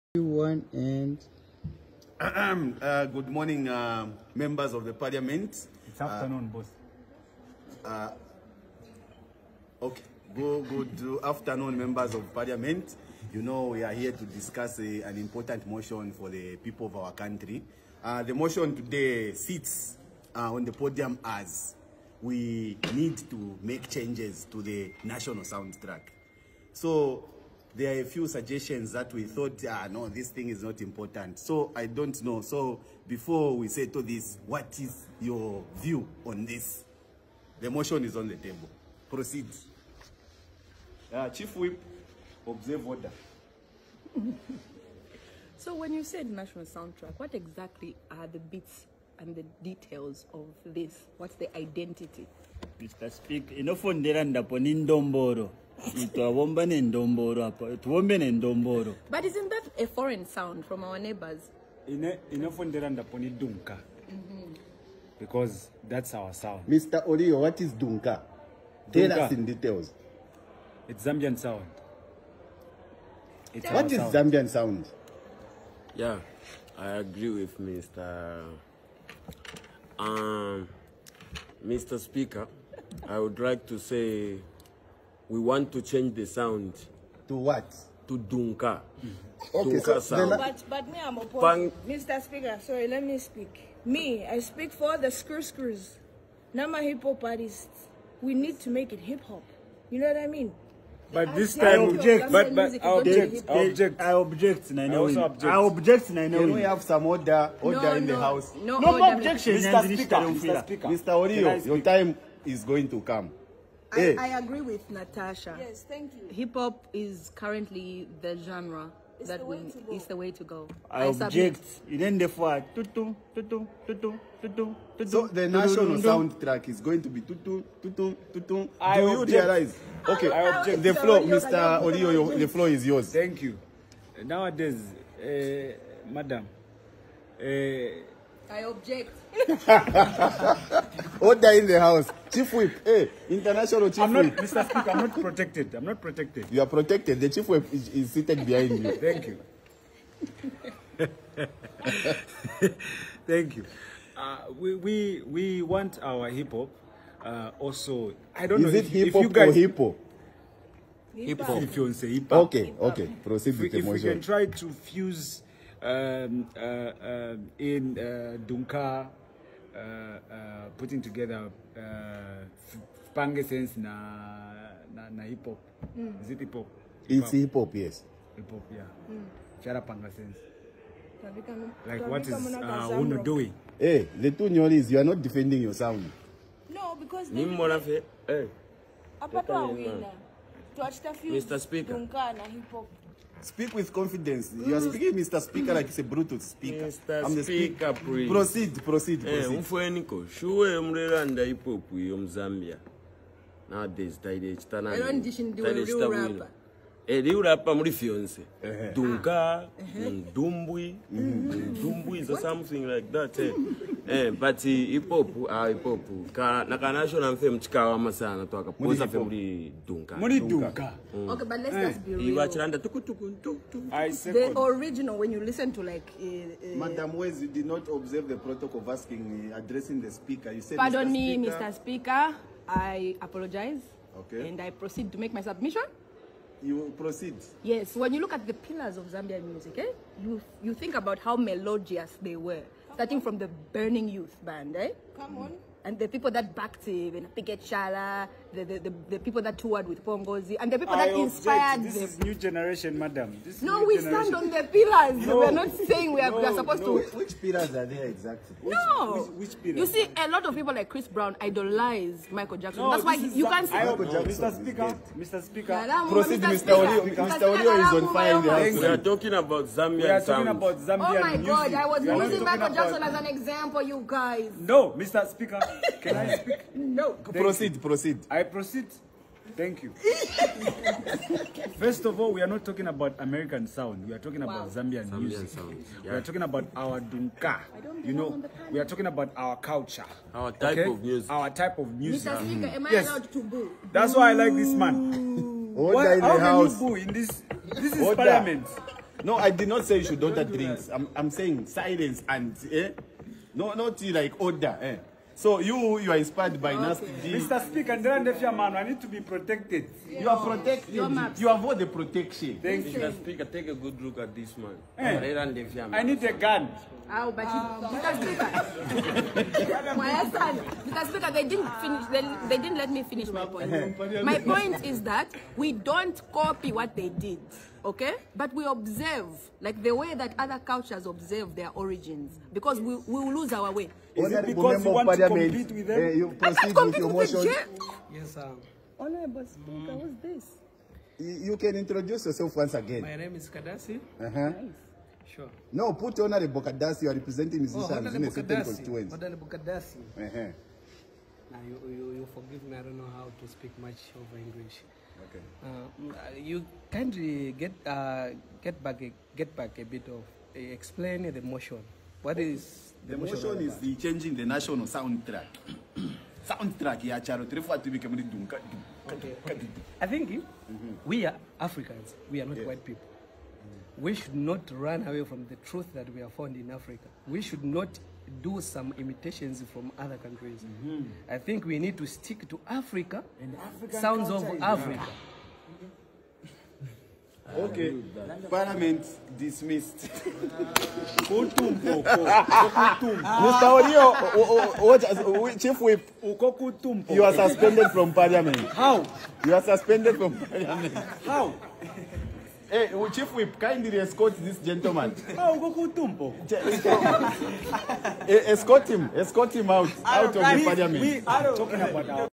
Zambian Music Parliamentary session-Sound Development
Zambian artists gather to discuss sound development where they talked about Dunka and Dombolo sounds. It features Jerayo Inc(speaker) Yo maps, Natasha Chansa, Chanda Na Kay, Wezhi and Dj Kandeke.

credits: Jerayo Inc